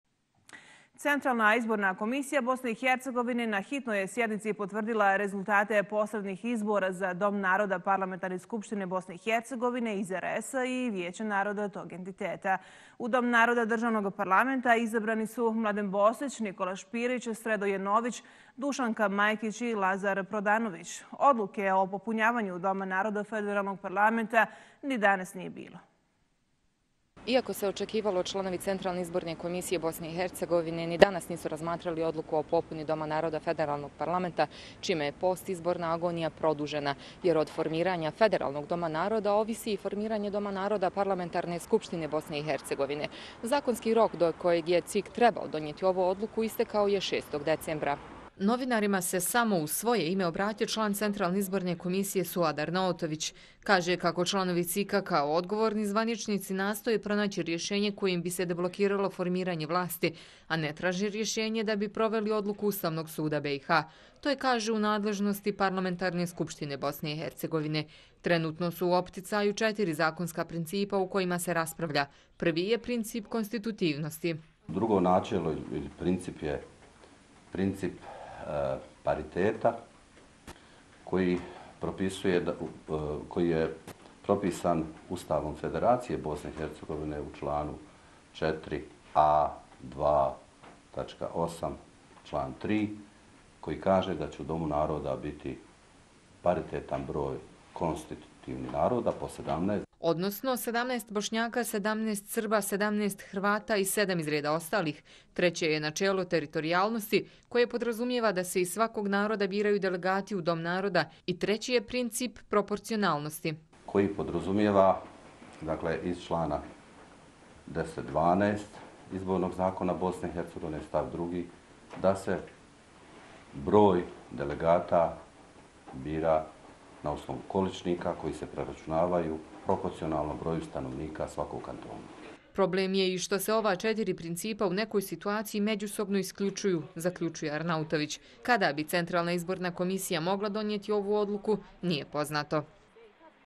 CIK NA STOLU IMA ČETIRI PRINCIPA U VEZI S POPUNOM DOMA NARODA (11 12 2018)
Centralna izborna komisija BiH na hitnoj je sjednici potvrdila rezultate posrednih izbora za Dom naroda Parlamentarne skupštine BiH iz RS-a i Vijeće naroda tog entiteta. U Dom naroda državnog parlamenta izabrani su Mladen Bosić, Nikola Špirić, Sredoje Nović, Dušanka Majkić i Lazar Prodanović. Odluke o popunjavanju Doma naroda Federalnog parlamenta ni danas nije bilo.